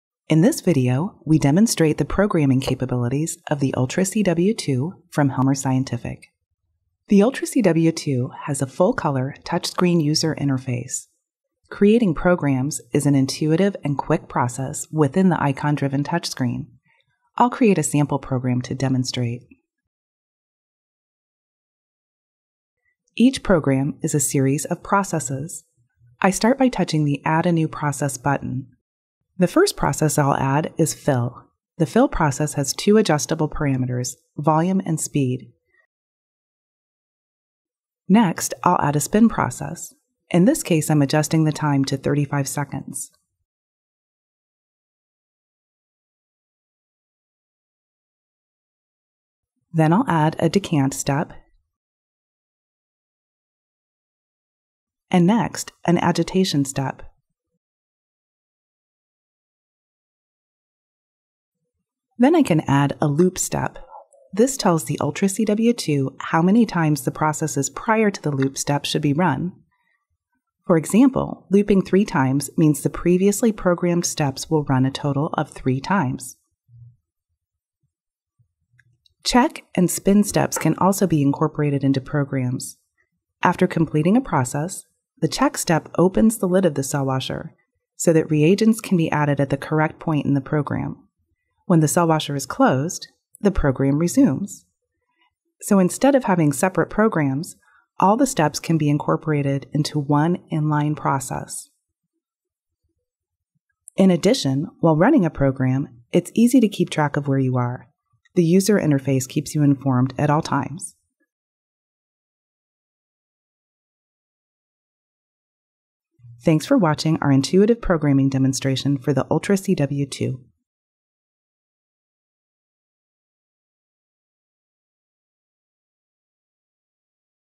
UltraCW® II Automatic Cell Washer: Intuitive Programming
- Create programs quickly with the easy-to-use touchscreen interface
- Incorporate check and spin steps into programs, making it easy to run wash cycles, add reagents, and perform a quick spin
- Create streamlined workflow with no need to transfer tubes to separate centrifuge for spin